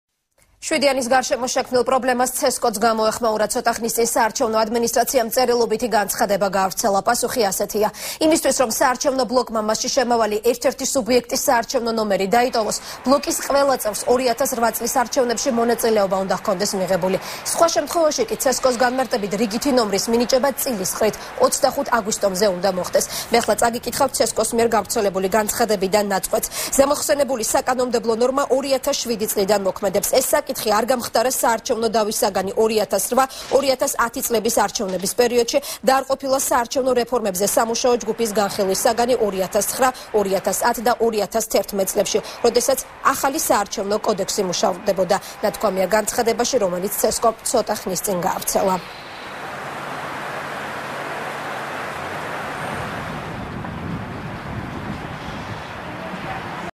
ახალი 3 | ცესკოს გამოხმაურება | 20.08.12
7 რიცხვზე შექმნილ პრობლემას ცესკოც ეხმაურება. ცოტა ხნის წინ საარჩევნო ადმინისტრაციამ წერილობითი განცხადება გაავრცელა. პასუხი ასეთია-იმისთვის, რომ საარჩევნო ბლოკმა მასში შემავალი ერთ-ერთი სუბიექტის საარჩევნო ნომერი დაიტოვოს, ბლოკის ყველა წევრს 2008 წლის არჩევნებში მონაწილეობა უნდა ჰქონდეს მიღებული.სხვა შემთხვევაში კი, ცესკოს განმარტებით, რიგითი ნომრის მინიჭება წილისყრით 25 აგვისტომდე უნდა მოხდეს.